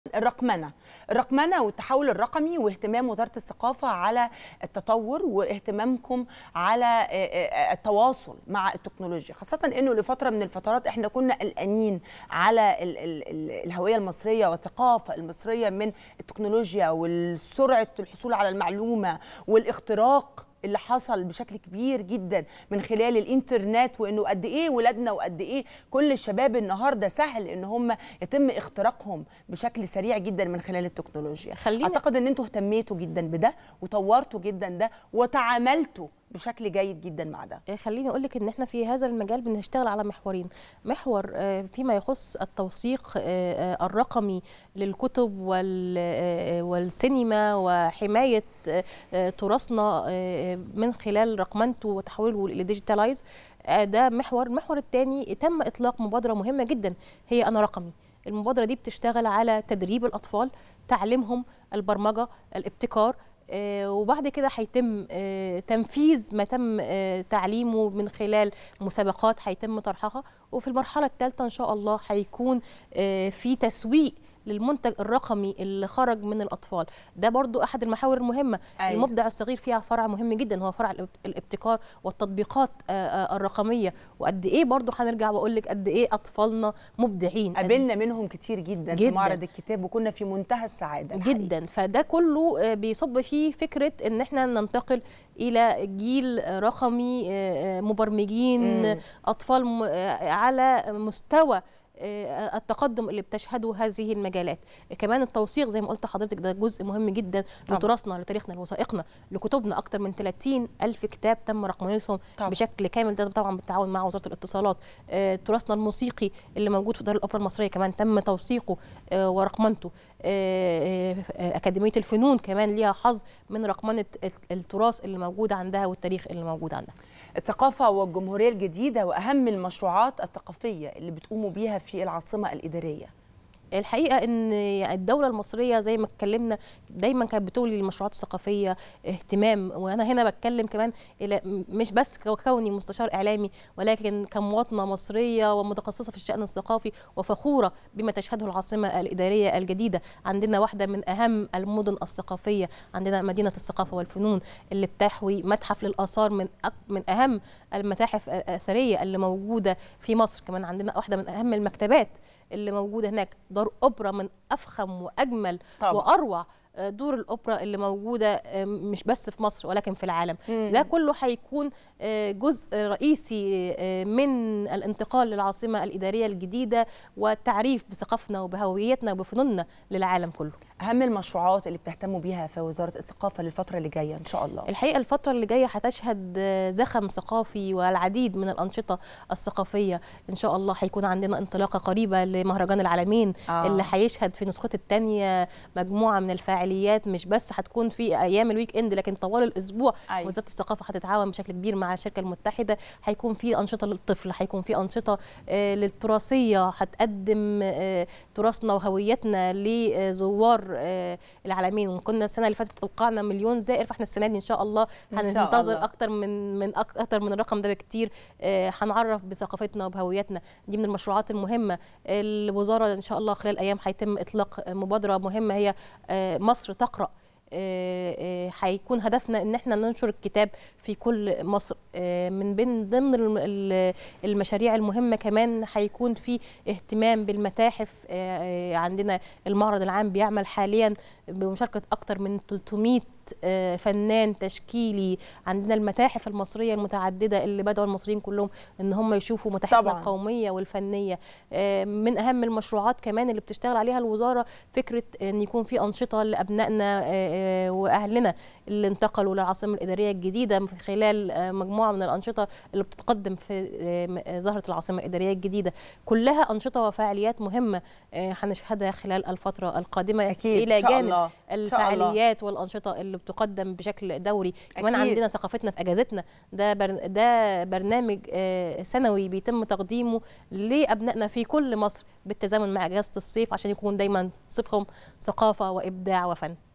كيف اهتمت وزارة الثقافة بالرقمنة والتحول الرقمي؟ المستشار الإعلامي لوزارة الثقافة رضوى هاشم تجيب
يحكى أن | كيف اهتمت وزارة الثقافة بالرقمنة والتحول الرقمي؟ المستشار الإعلامي لوزارة الثقافة رضوى هاشم تجيب